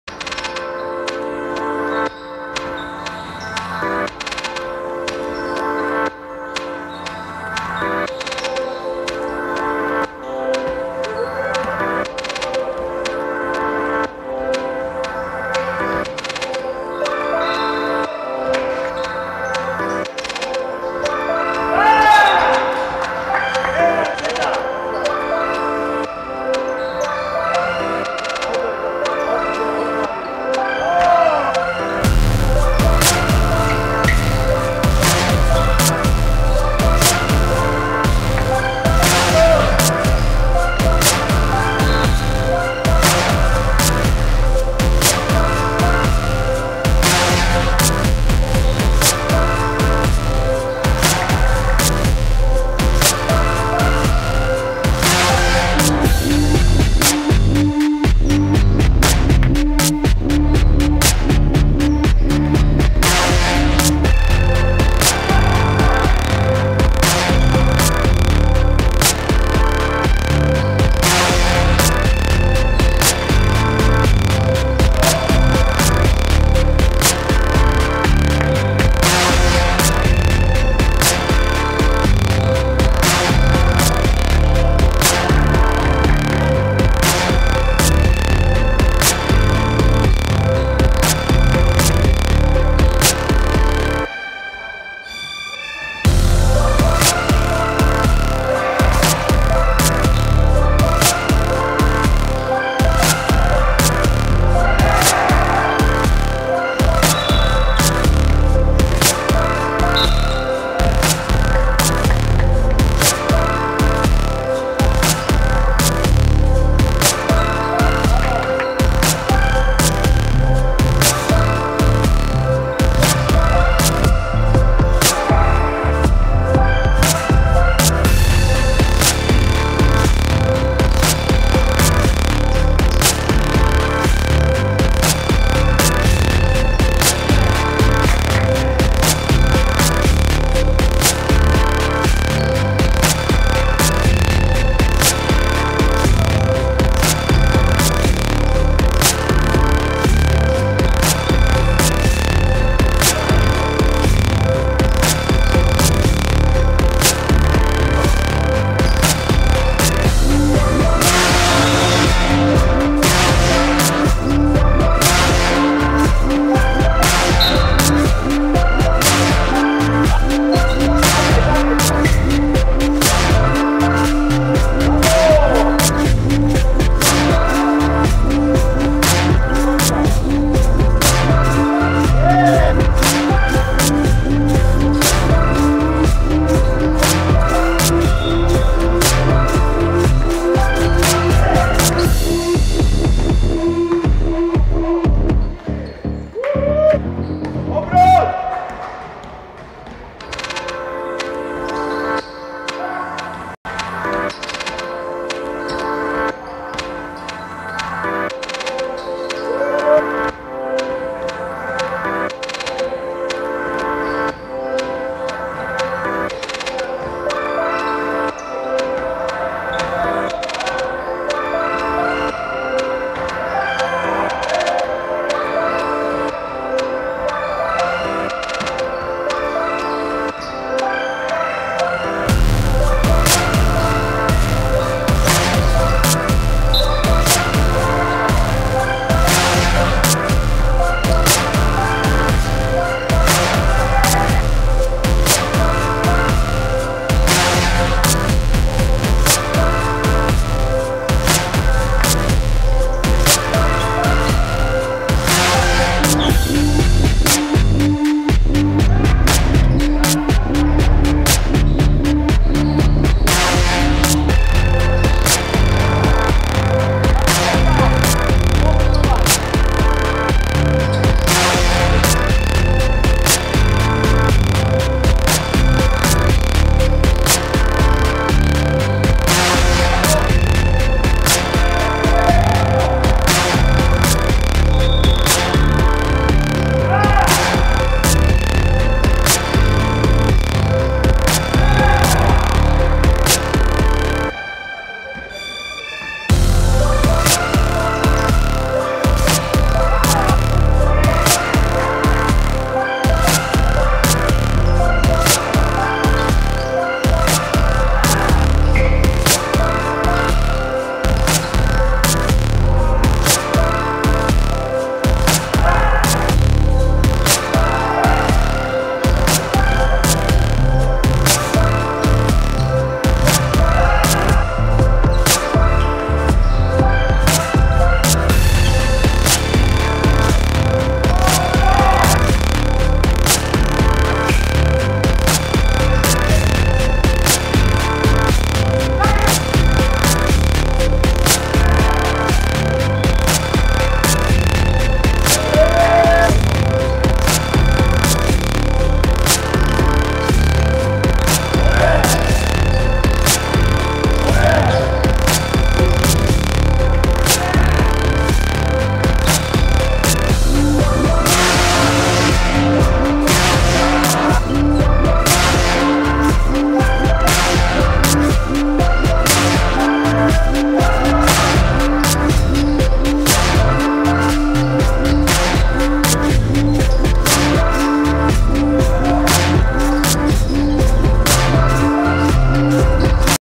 14 11 Portland vs Los Angeles – Basket 36+ Niedziela
PORTLAND 70:65 LOS ANGELES
A.Wójcicki 24pkt 7zb 7ast
T.Gerung 17pkt 9zb